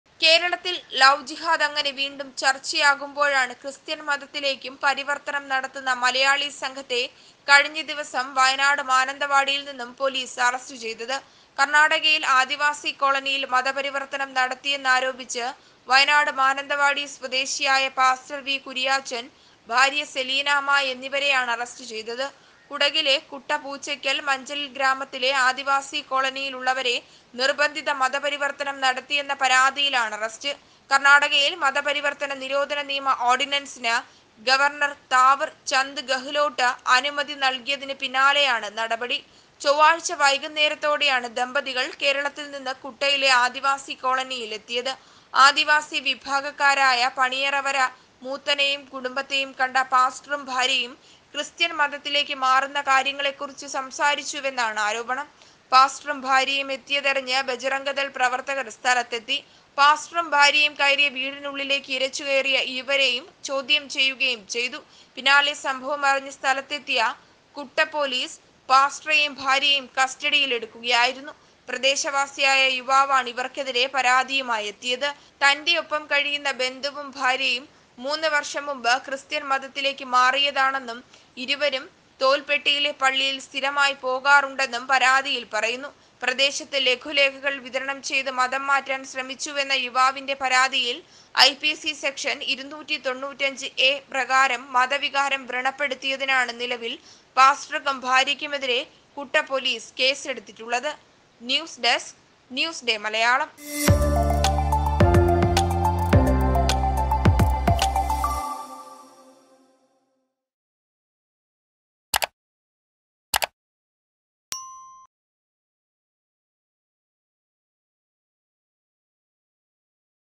കേസെടുത്ത് പോലീസ് | NEWS DAY MALAYALAM
മതം മാറ്റാന്‍ ശ്രമം; ബജ്‌റംഗ് ദള്‍ എത്തി

കേസെടുത്ത് പോലീസ്